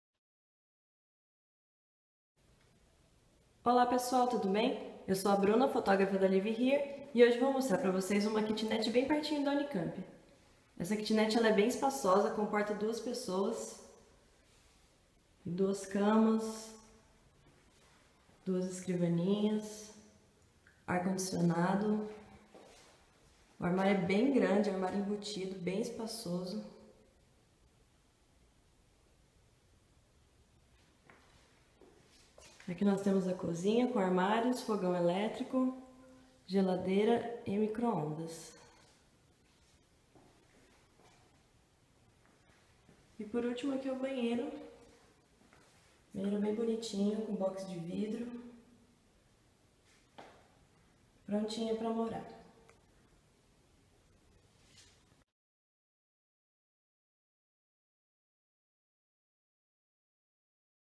[K180122312] Kit 5
Kitnets planejadas, recém construídas, localizadas entre duas das principais avenidas de Barão Geraldo, e a apenas 400 metros do Restaurante Universitário da Unicamp. Ambiente amplo, possui lavanderia compartilhada, cozinha interna em cada kitnet, ar condicionado e possibilidade de utilizar vaga na garagem (sob consulta, por kitnet). Disponibilidade para até dois moradores por quarto. /